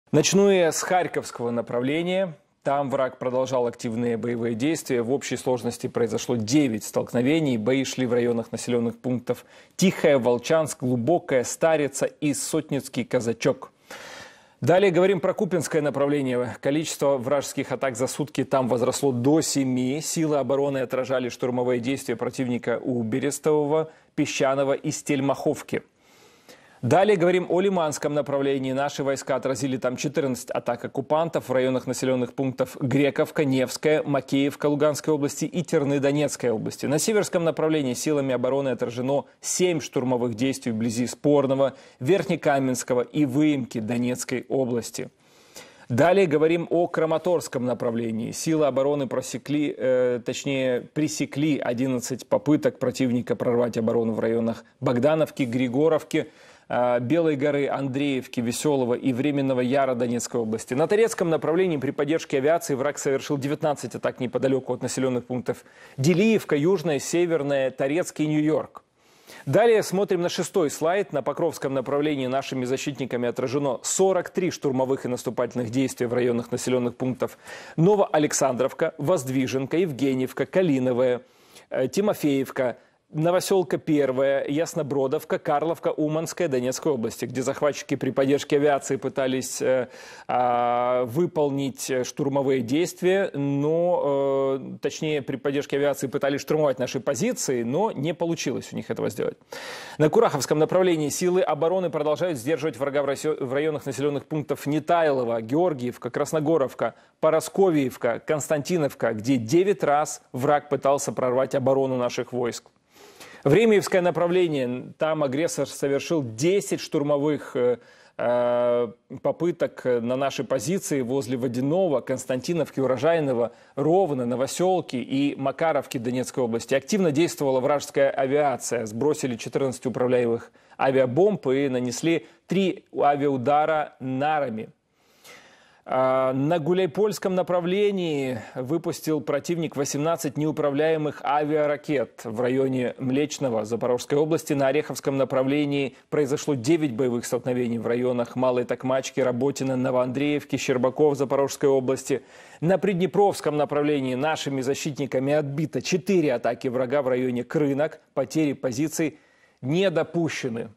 🔥 Карта войны: РФ пытается ПРОРВАТЬ оборону ВСУ – все ТЩЕТНО! Потери позиций НЕ ДОПУЩЕНЫ
На Харьковском направлении произошло 9 боевых столкновений. Силы обороны Украины успешно отразили атаки и на Лиманском, Северском, Краматорском направлениях. На Торецком направлении, при поддержке авиации, РФ совершила 19 атак. На Покровском – ВСУ отразили 43 штурмовых и наступательных действия врага.

Больше деталей – в рубрике "Карта войны".